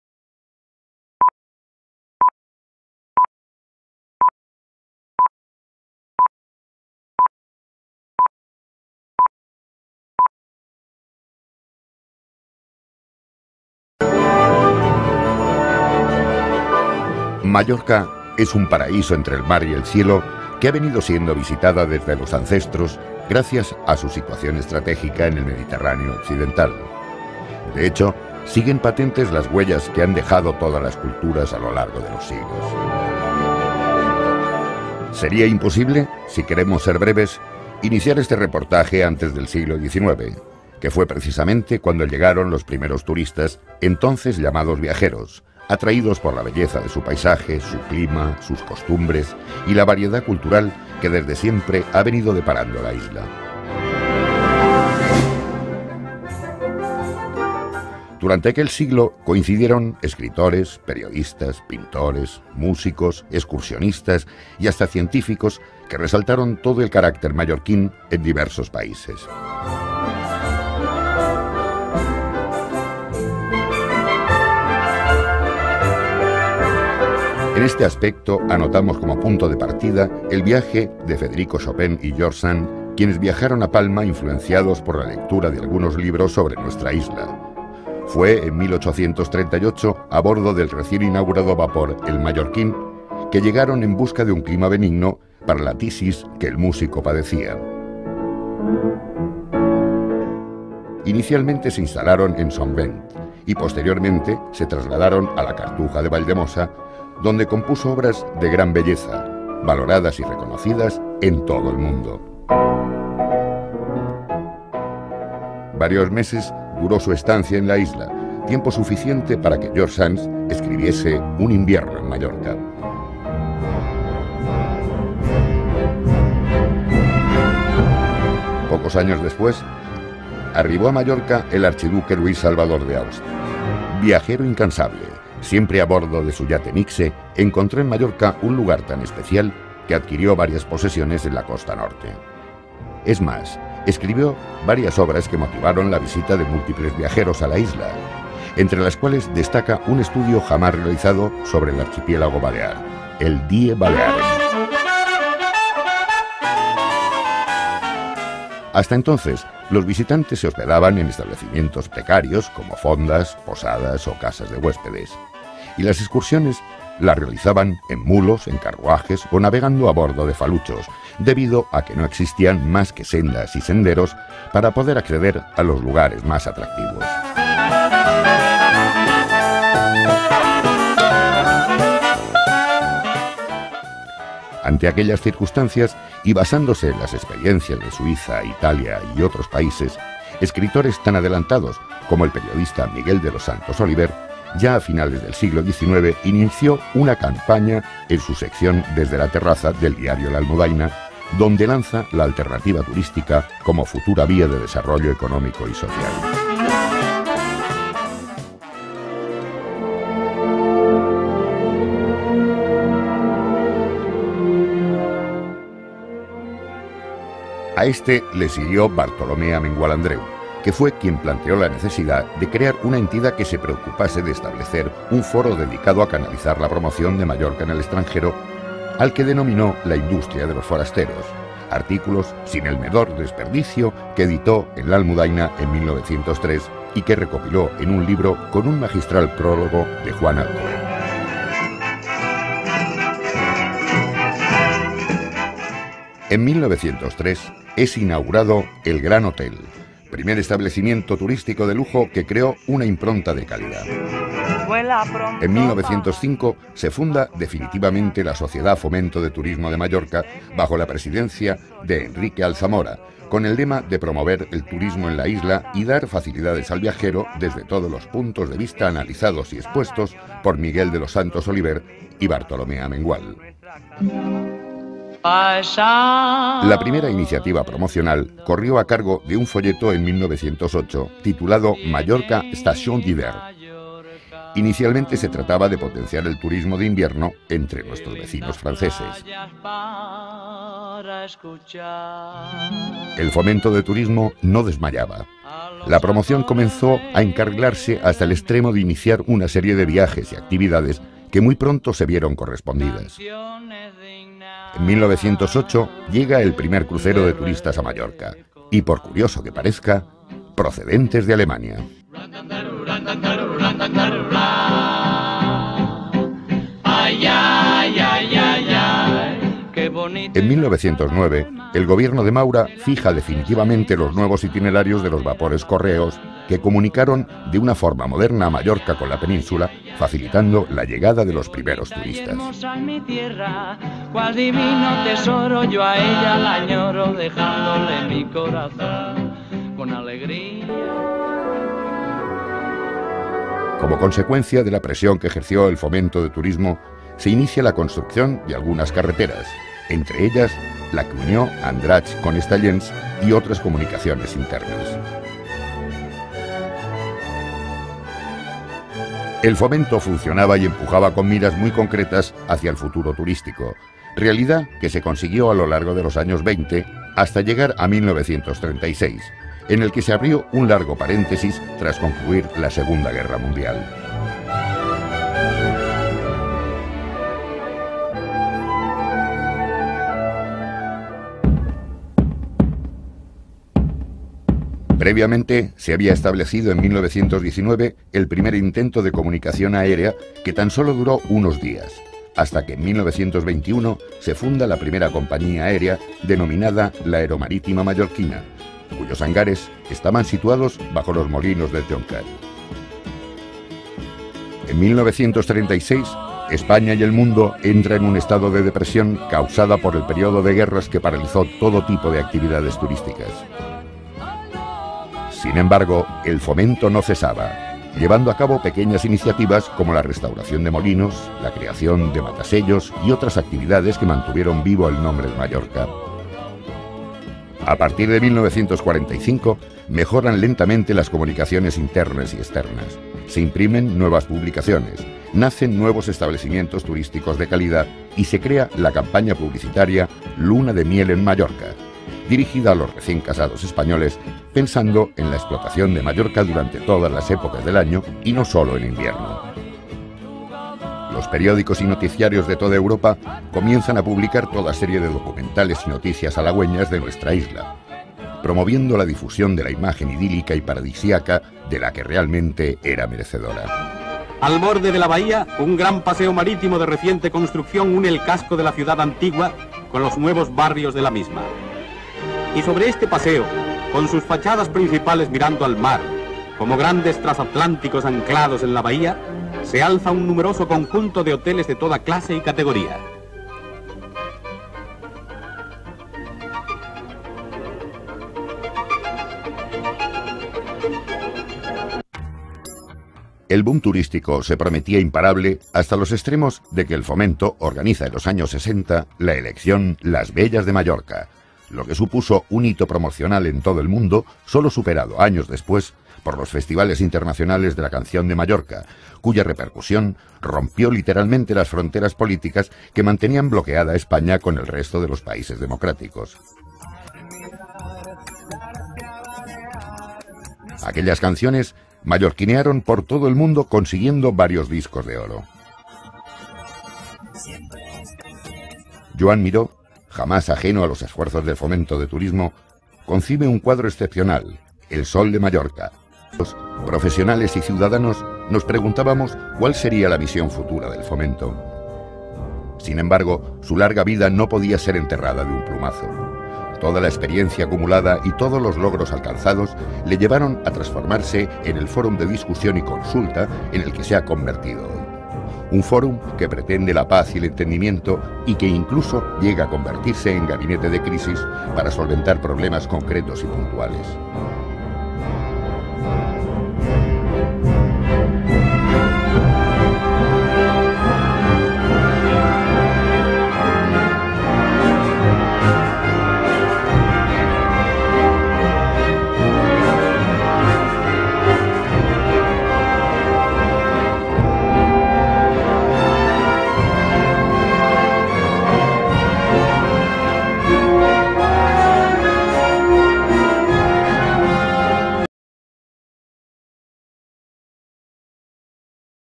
Centenario del Fomento del Turismo en Mallorca 1905-2005
Historia del Fomento con imagenes antiguas y modernas de Mallorca